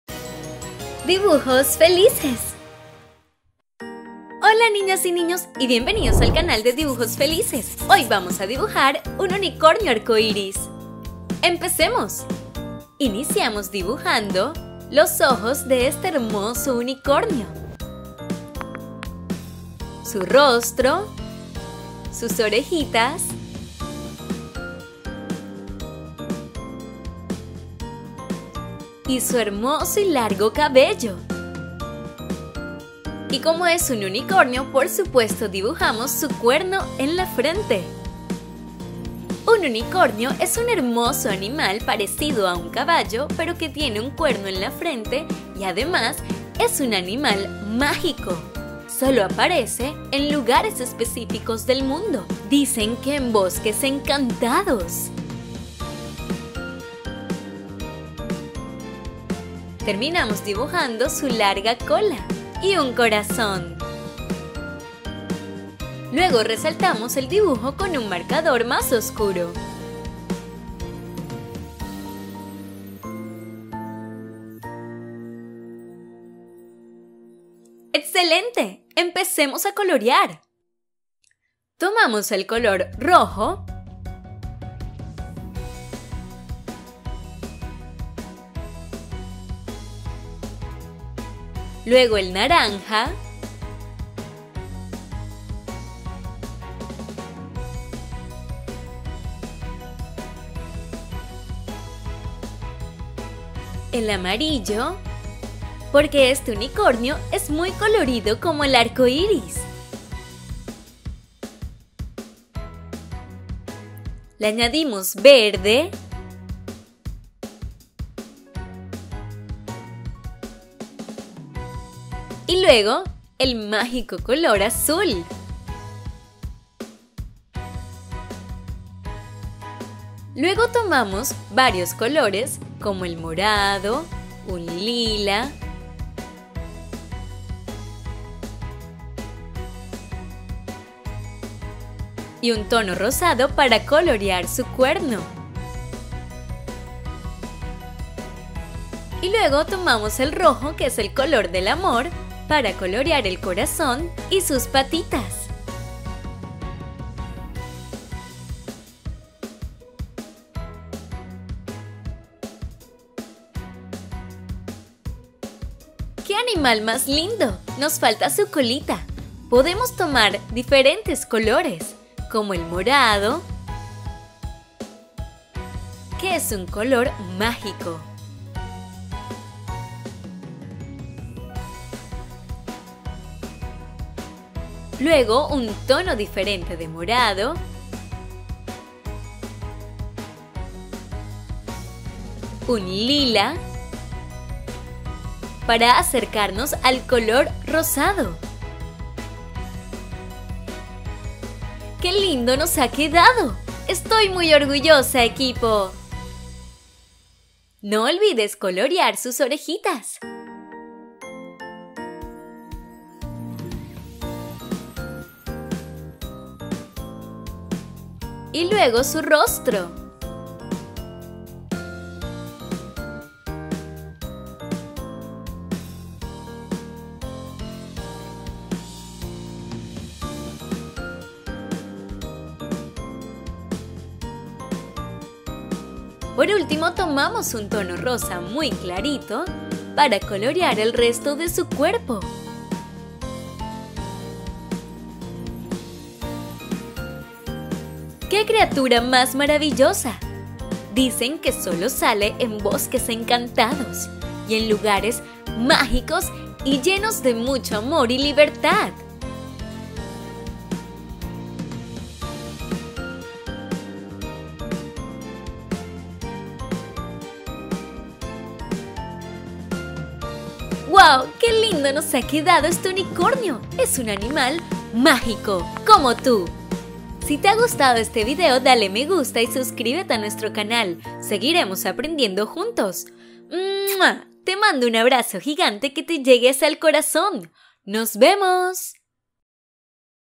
Cómo Dibujar y Colorear Un Unicornio Arcoiris 🦄🌈 Dibujos Para Niños
Hola, ¡en este video te muestro Cómo Dibujar y Colorear Un Unicornio Arcoiris 🦄🌈!
Tutorial de dibujo de Unicornio Para Niños